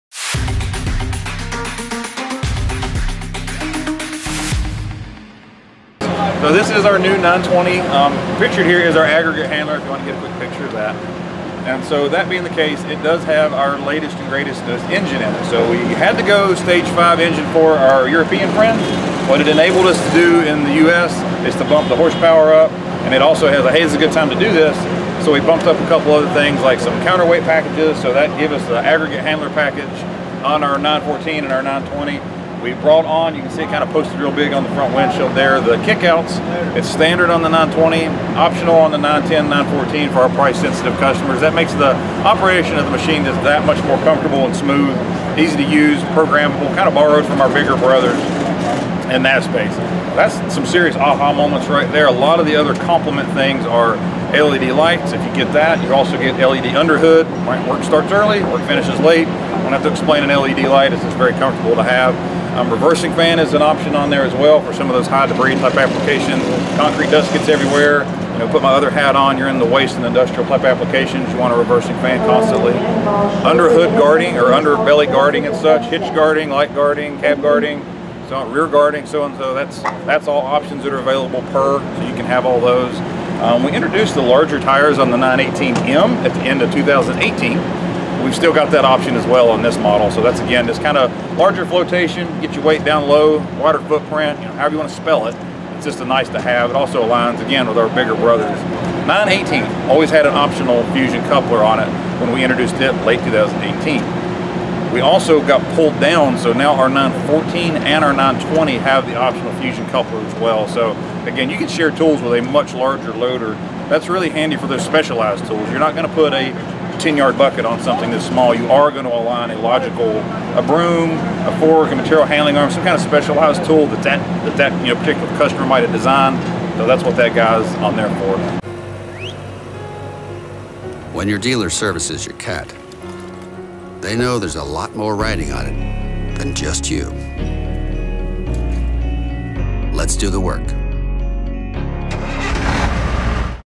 Caterpillar 920 Aggregate Handler Meets Stage 5 Requirements
With Stage 5 engine requirements, Caterpillar was able to boost engine horsepower up to 120 hp. Additional upgrades include bumped up counterweight packages and kickouts.

Additional optional features include LED lights, reversing fan, guarding, larger tires and more.